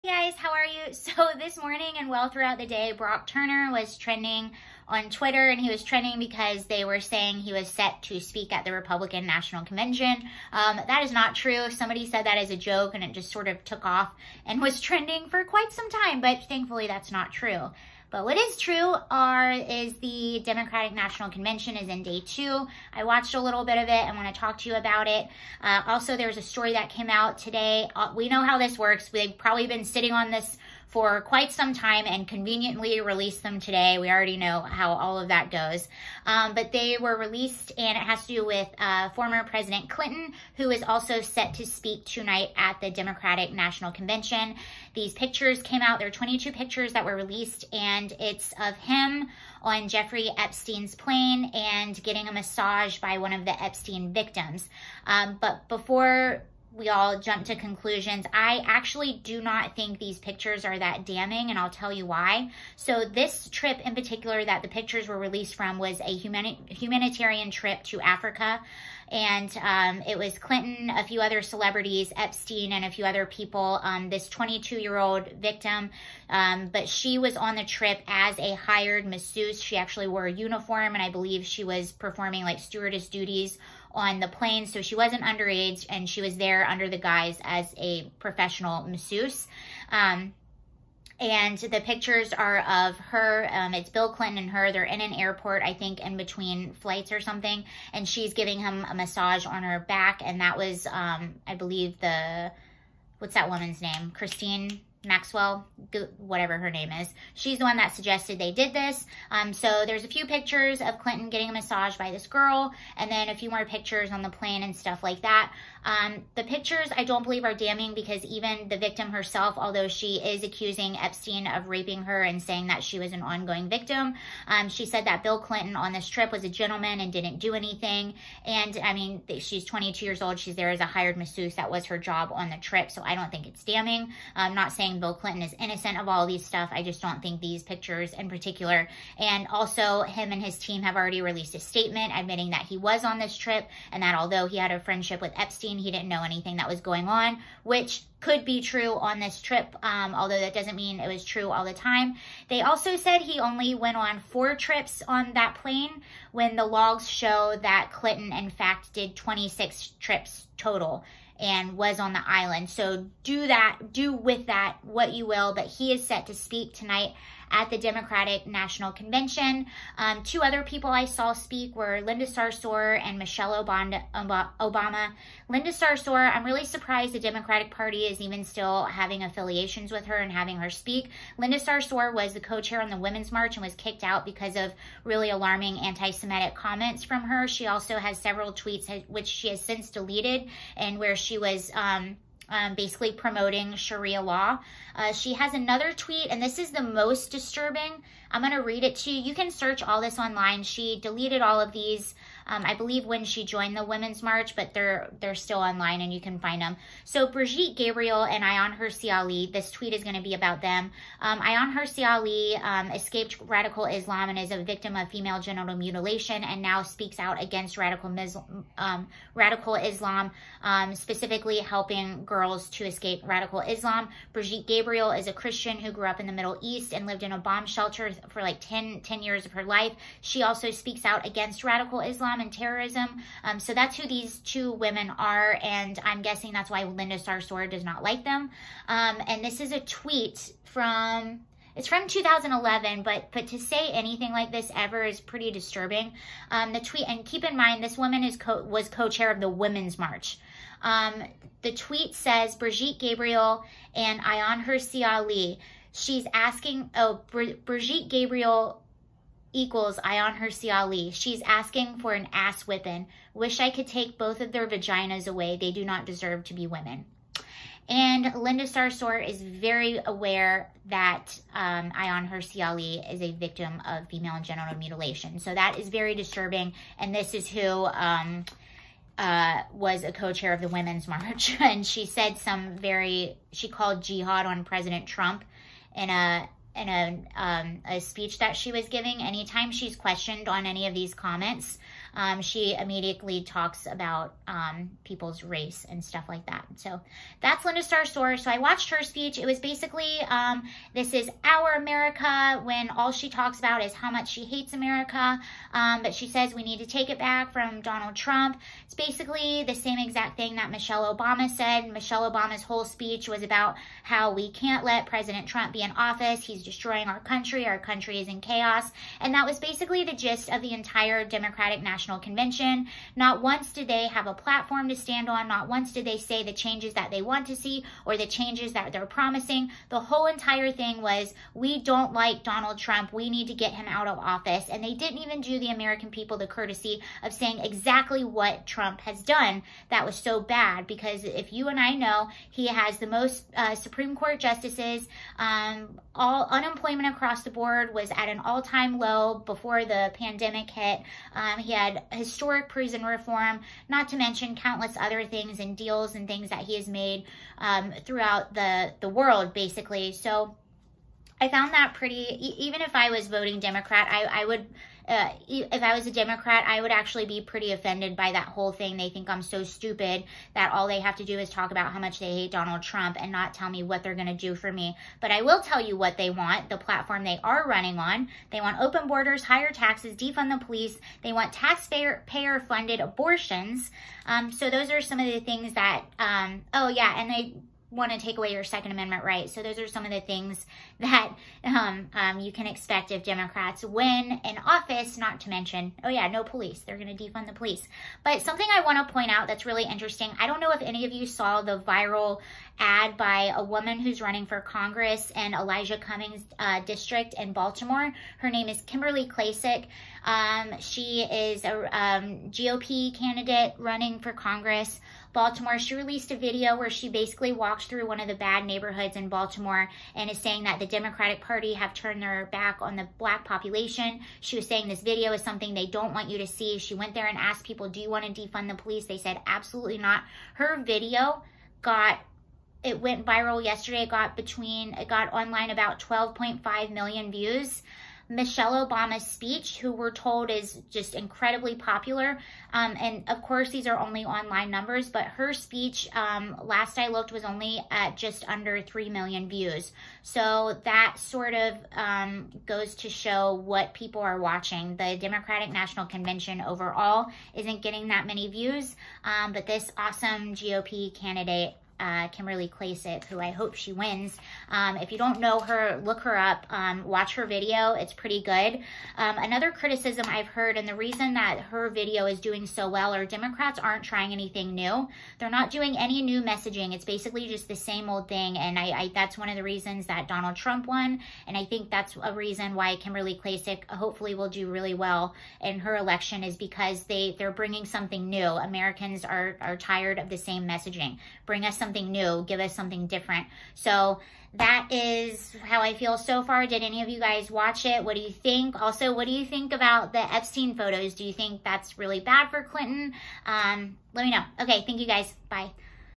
Photos Of Clinton Released On Eve Of Speaking At DNC, Michelle Obama and Linda Sarsour Also Speak
The DNC is in day 2 now and here are my thoughts now.